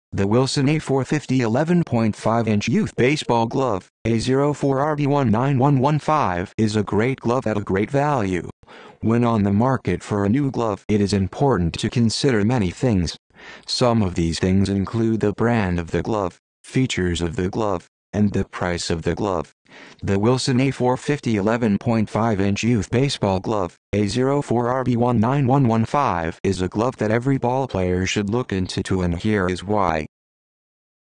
Rawlings Quatro Pro USSSA Baseball Bat Drop 12 UT9Q12 | Baseball Bargains
The Rawlings Quatro Pro USSSA Baseball Bat Drop 12 UT9Q12 has great pop and is built to have a long lasting feel.